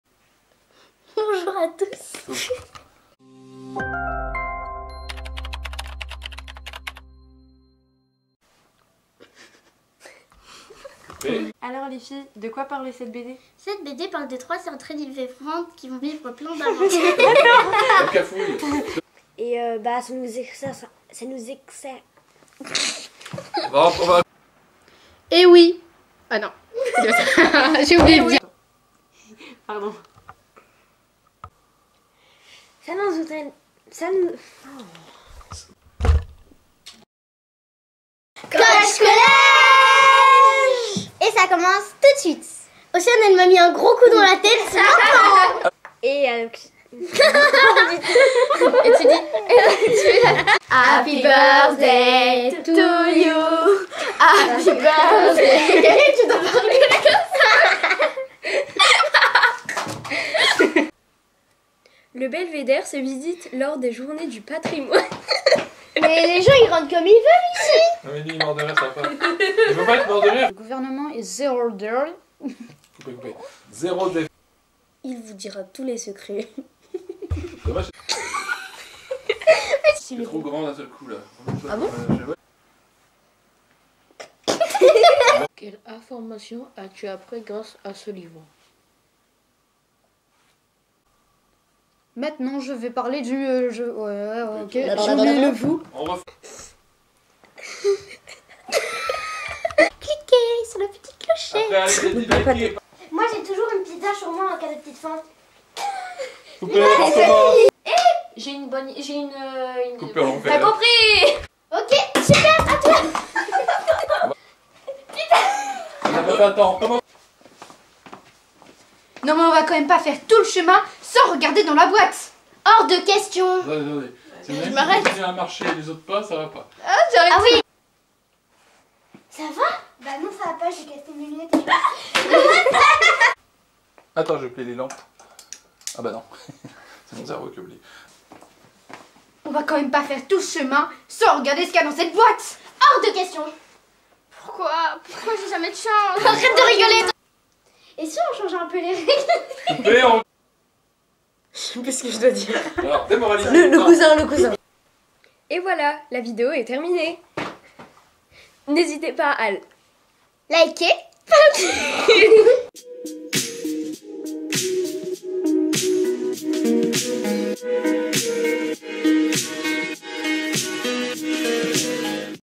BETISIER PAQUES 2023
Comme chaque année depuis la naissance de la Web TV en 2014, voici le très attendu bêtisier de Pâques. Quelques erreurs et beaucoup de rigolades sont au programme.